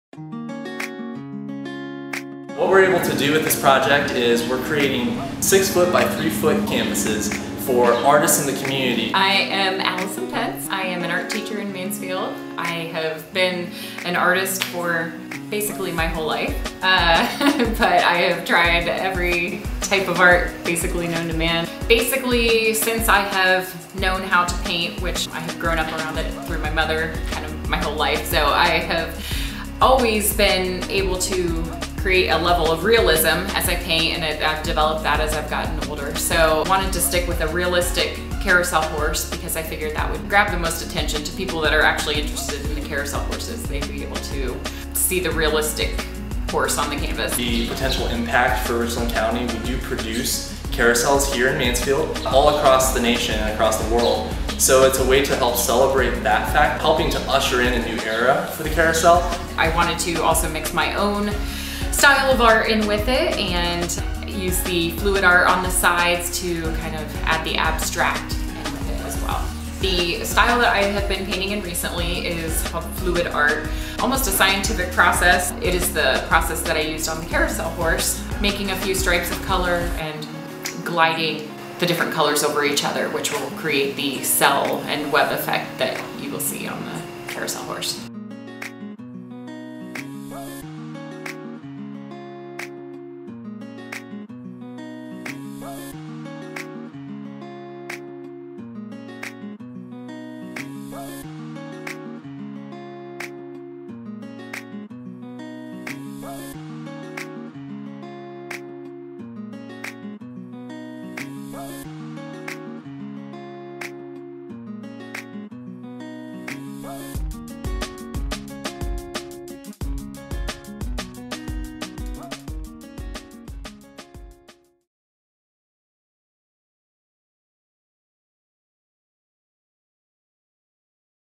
Richland Carrousel Popup Mural Showcase ~Allison Pence +Mankind Murals Artist Spotlight
Mankind Murals has created a project with Richland Carrousel Park, depicting each individual Horse and Animal on a large 6ftx3ft canvas.

Allison Pence, local artist and art teacher, shares some of her story and technique.  By capturing both realism and abstraction in her original canvas, Pence creates something entirely unique for this one-of-a-kind Carrousel Mural Showcase.